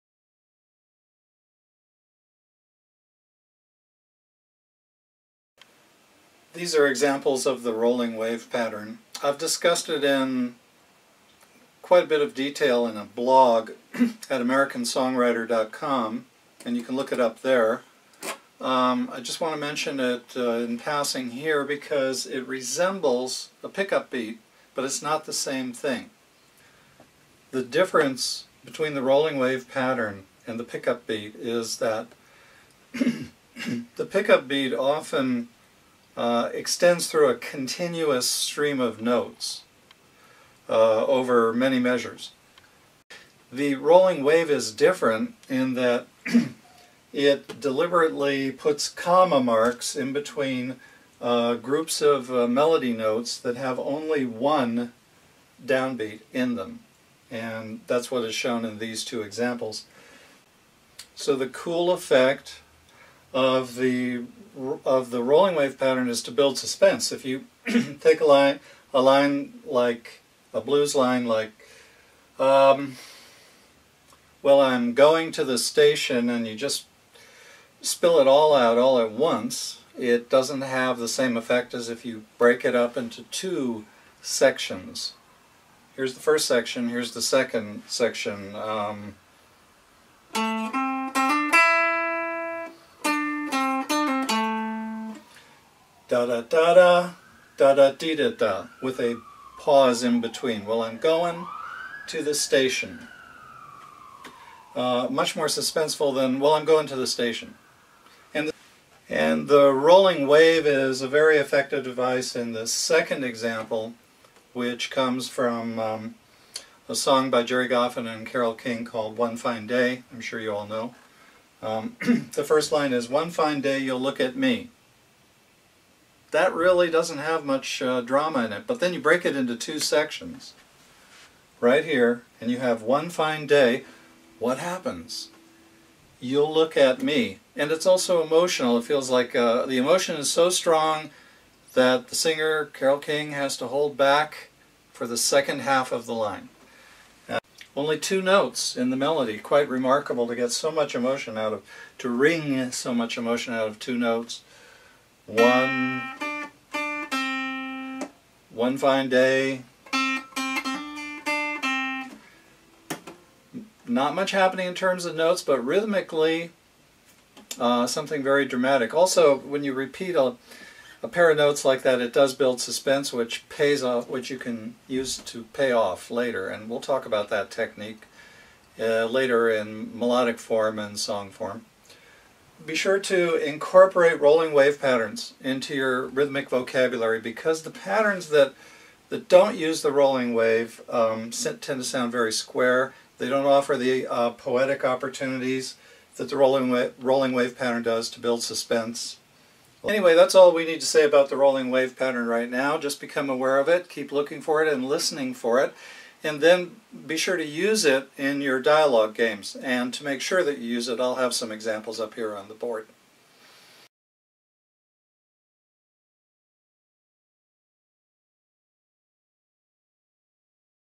Songwriting ABCs - Video 1.20 - "The Rolling Wave Rhythm"
A "rolling wave" is a group of melody notes that cluster around the downbeat, with rests before and after, which creates a telegram-like effect: short bursts of imagery and feeling that generate suspense. They are usually found in two-measure sections, which are the typical building blocks of pop songs and blues.

Rolling waves are extremely common throughout pop music of the last sixty years (they sound good when you're cruisin' down the highway), so when I first noticed them cropping up over and over again in hit song after hit song, it was a bit of a shock to discover that -- as far as I could tell -- nothing had been written about them. I decided to call them a rolling wave rhythm because of the peak in the center (the downbeat), and the troughs in front and behind (the rests). By the way, those "troughs" are great places for the band to step in with great riffs and a solid beat. All-in-all, the rolling wave is a great songwriting "trick," as proven by its popularity.

Check my blog at americansongwriter.com for more.

Once you know what a rolling wave rhythm sounds like, listen for it in your favorite songs and add it to your rhythmic vocabulary.